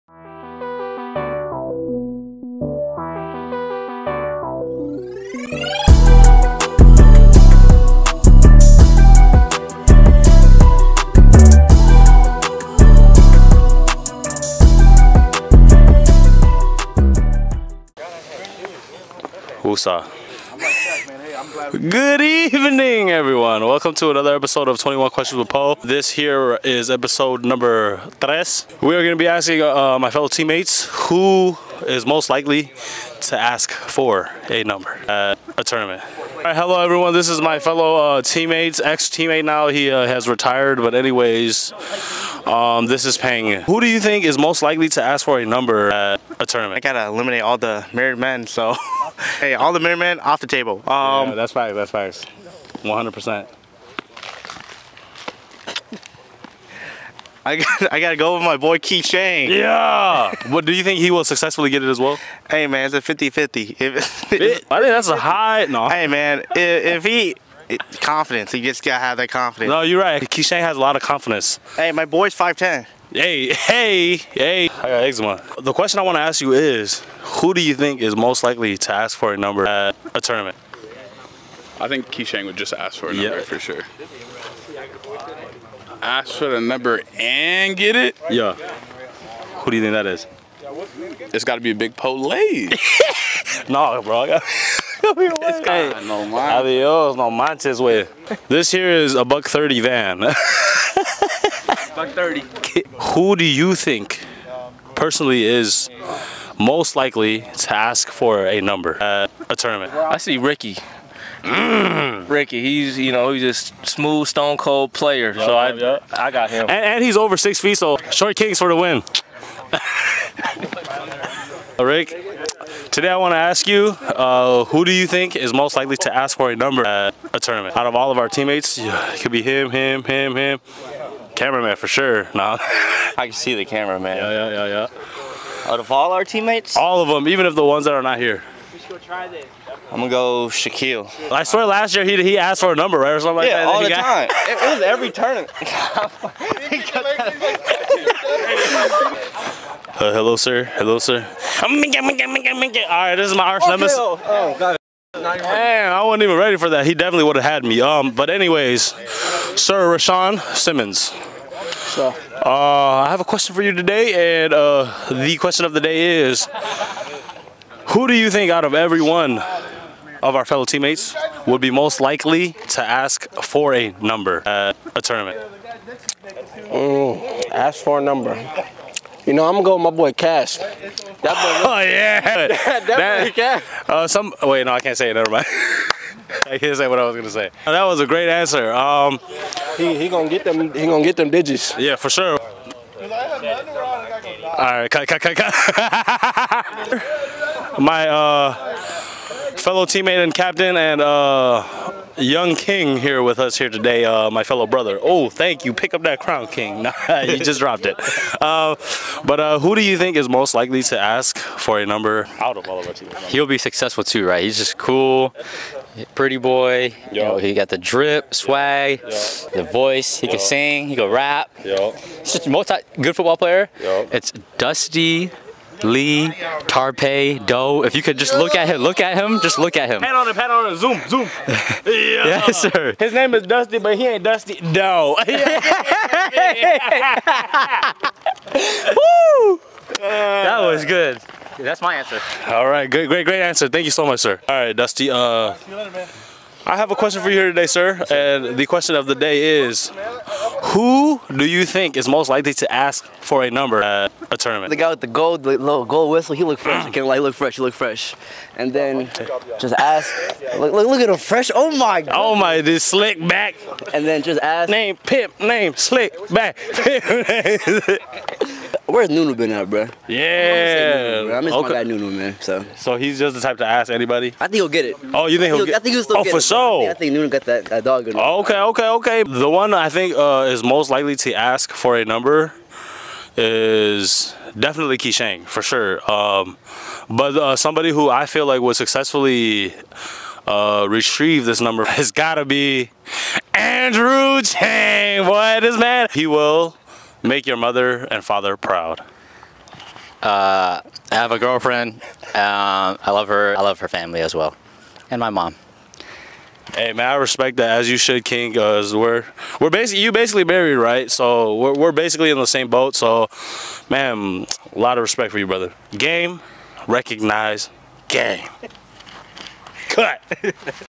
21 Questions (Ep. 3) - "Who is Most Likely to Ask for a Number at a Tournament?"
On the third episode of 𝟐𝟏 𝐐𝐮𝐞𝐬𝐭𝐢𝐨𝐧𝐬 𝐰𝐢𝐭𝐡 𝐏𝐡𝐨,
Pho Lay Htoo asks fellow Witness teammates which Witness player they believe would ask for a number at a sports tournament.